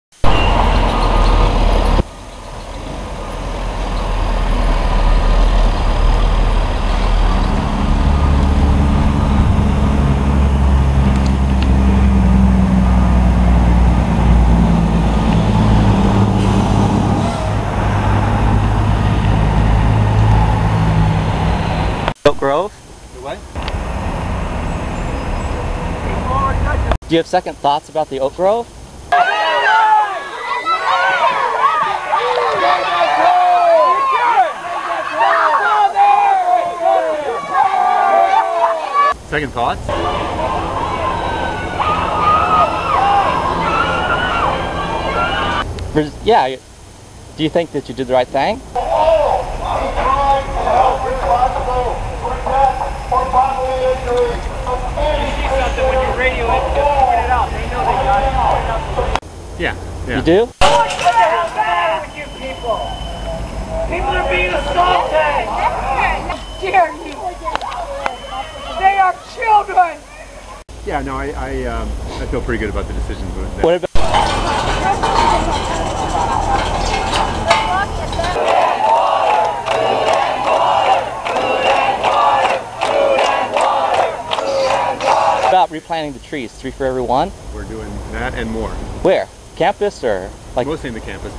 Nathan Brostrom: A Digital Short
Murder of trees, attempted-murder of Tree-sitters, and UC Berkeley Administrator Nathan Brostrom... did he do the right thing?